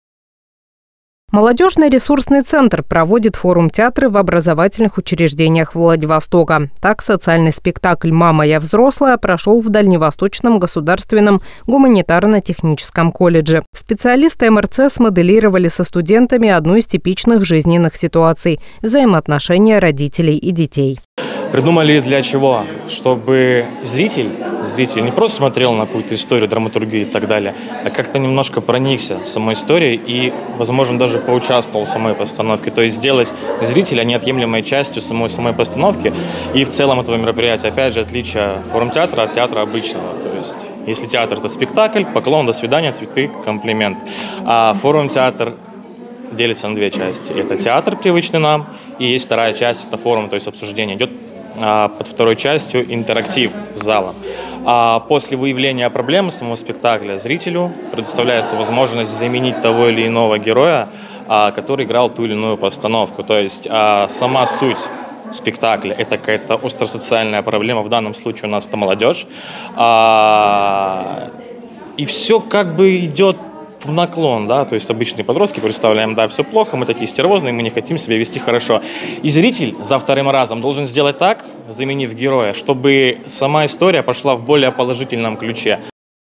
Форум-театры.
Молодежный ресурсный центр проводит форум-театры в образовательных учреждениях Владивостока. Так, социальный спектакль «Мама, я взрослая!» прошел в Дальневосточном государственном гуманитарно-техническом колледже. Специалисты МРЦ смоделировали со студентами одну из типичных жизненных ситуаций – взаимоотношение родителей и детей.